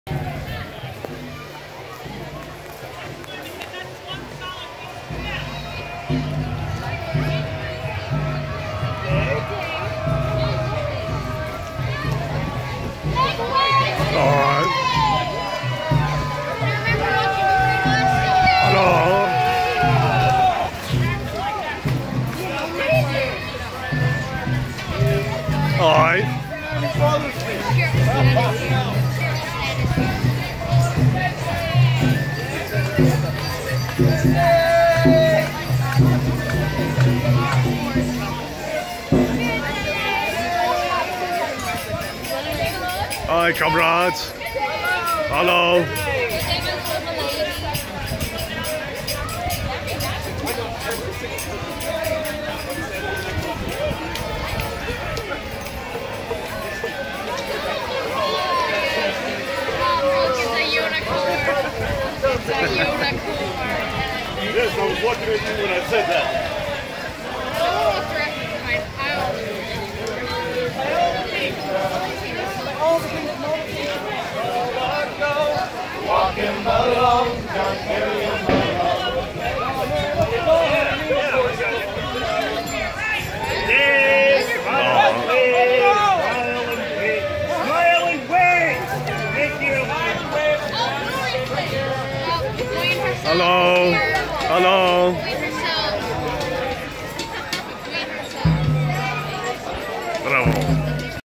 Wonderful Parade
I have traveled to the town Shire of Willy Nilly . And this was my first time at this beloved and wonderful Ohio Rennaisance Festival.  It was very fun and very impressive.  Very large and magical shire indeed.